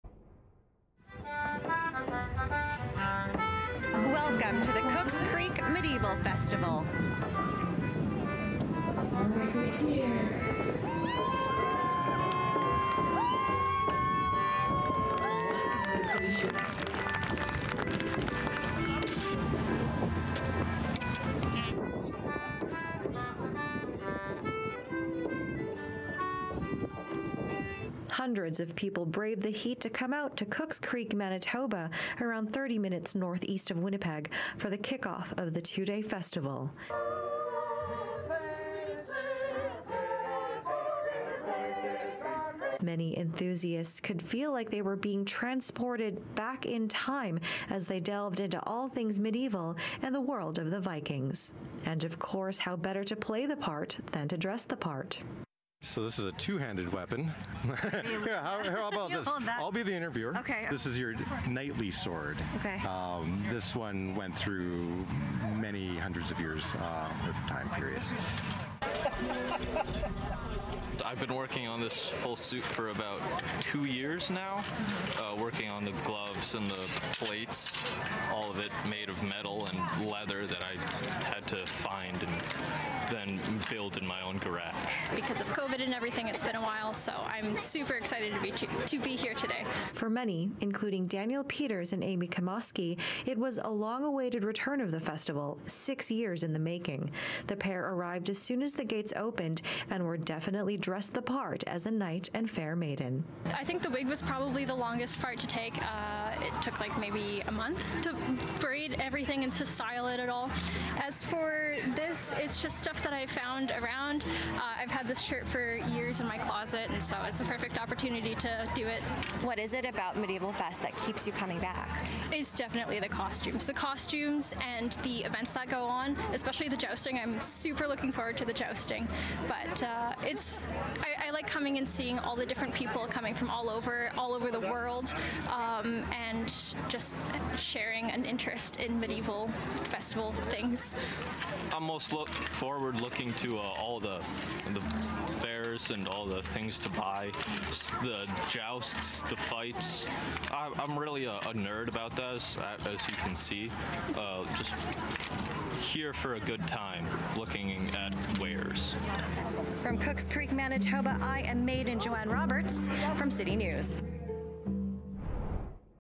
Hundreds kick-off medieval festival in Manitoba
Knights, maidens and nobles... After a 6 year hiatus, Cooks Creek welcomed back its medieval festival to hundreds of enthusiasts Saturday. Joanne Roberts has the story.